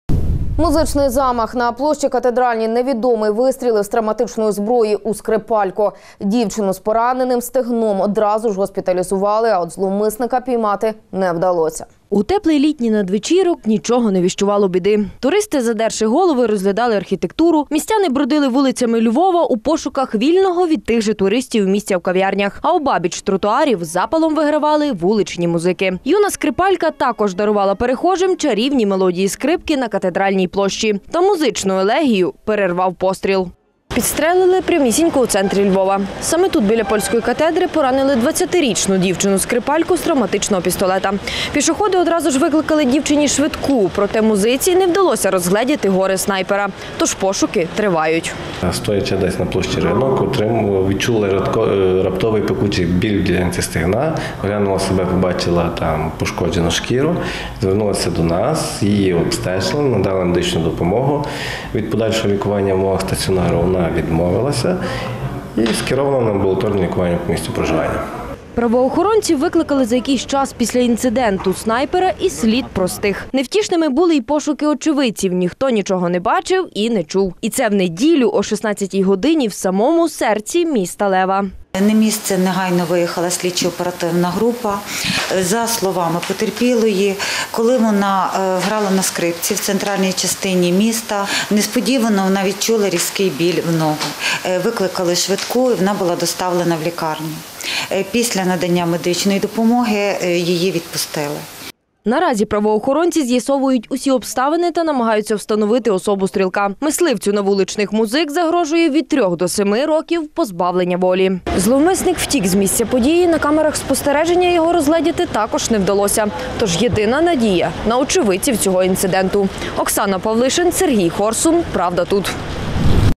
Музичний замах. ПравдаТУТ Львів
Музичний замах. На площі Катедральній невідомий вистрілив з травматичної зброї у скрипальку. Дівчину із пораненим стегном одразу ж госпіталізували. А от зловмисника піймати не вдалося.